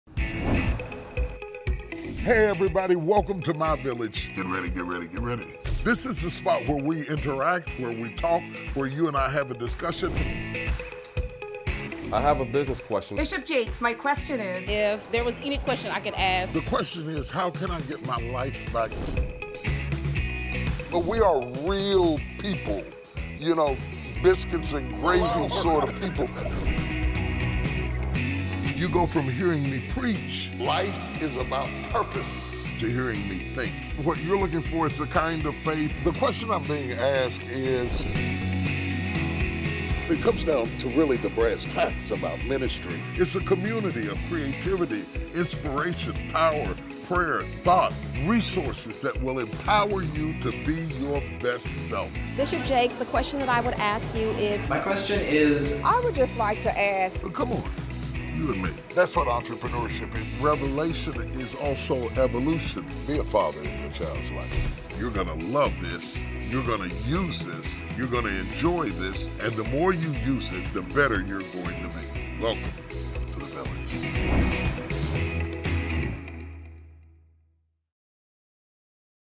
T.D. Jakes Introduces Bishops Village
Go beyond Sunday’s sermons. Get access to exclusive video chats with T.D. Jakes where you ask the questions, and he delivers the answers.

T.D. Jakes was inspired to create this interactive community as a transformative, faith-based resource designed for you to become the absolute best you can be. Bishop’s Village will put you front and center with T.D. Jakes, providing you with valuable tools and insight so you can learn more, live more, and further your faith.

Don’t miss out on this exclusive opportunity. Get connected with Bishop’s Village today!

Access
sermons, conferences, and more exclusive content: